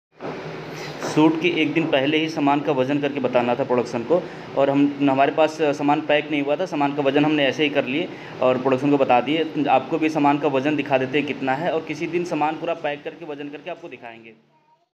Weight Machine | Arri Alexa Mini Full setup with Lenses and all Full weight | Camera Hub
in this video I am showing you
Arri Alexa Mini
Cook s4 i lenses
Sort Zoom
solder pad
haid
stand patala
all accessories of setup
weight for your knowledge for flight travel